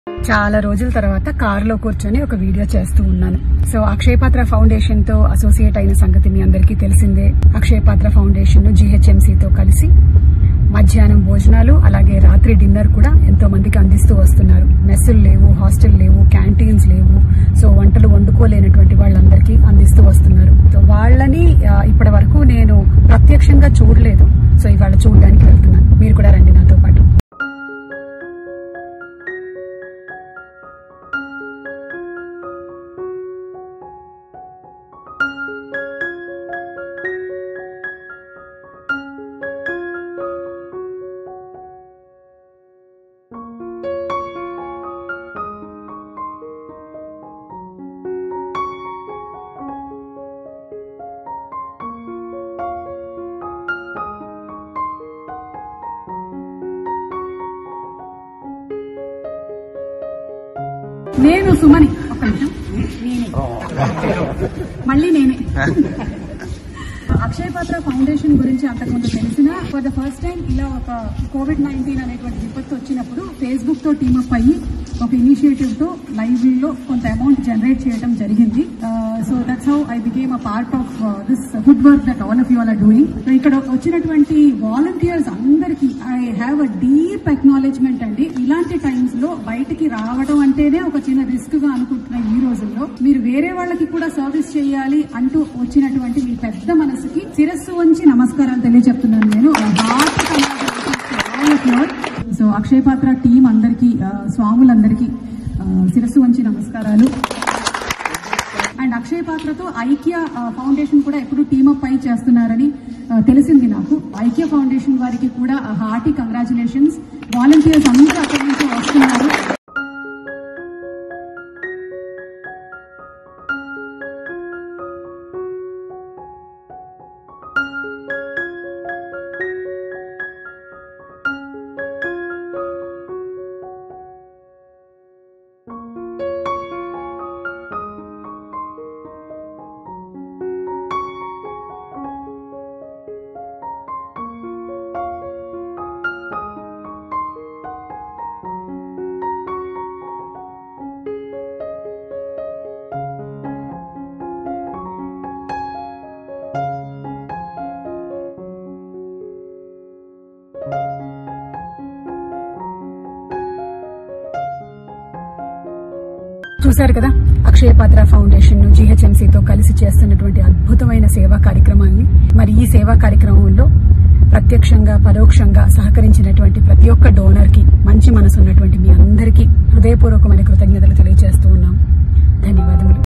Sumakka Appreciates Akshaya Patra Foundation For Serving The Poor Even Lockdown | Daily Culture
Sumakka Appreciates Akshaya Patra Foundation For Serving The Poor In Lockdown